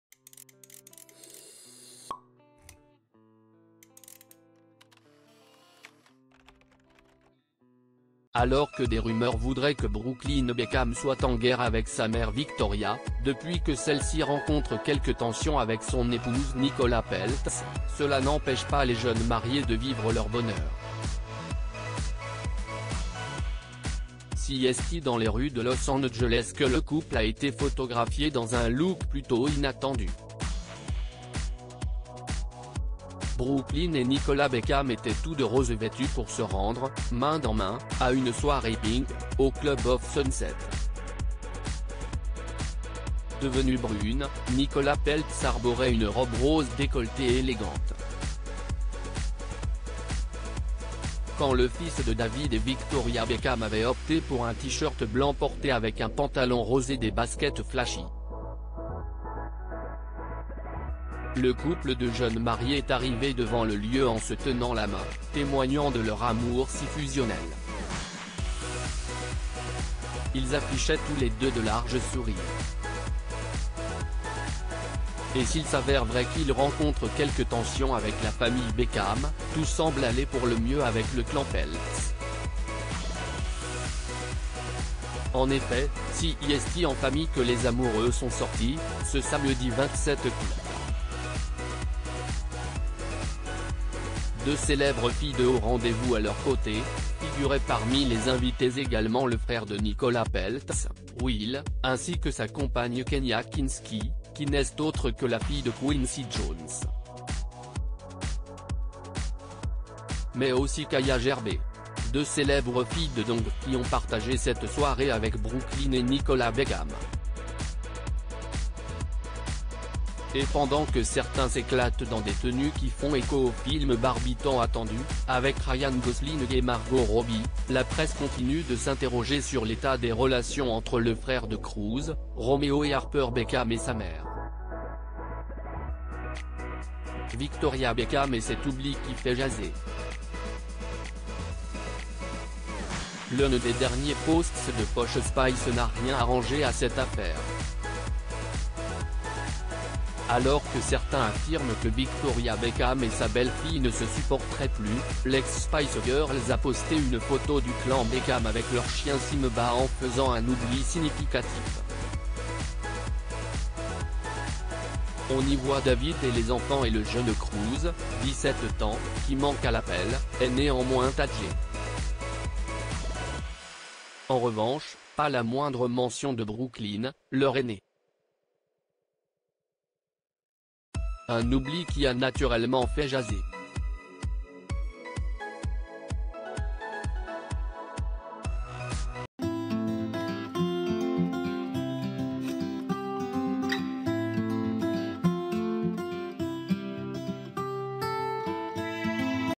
Brooklyn Beckham et Nicola Peltz en total look rose : Amoureux et assortis pour une soirée avec une
Brooklyn Beckham et Nicola Peltz en total look rose : Amoureux et assortis pour une soirée avec une célèbre fille de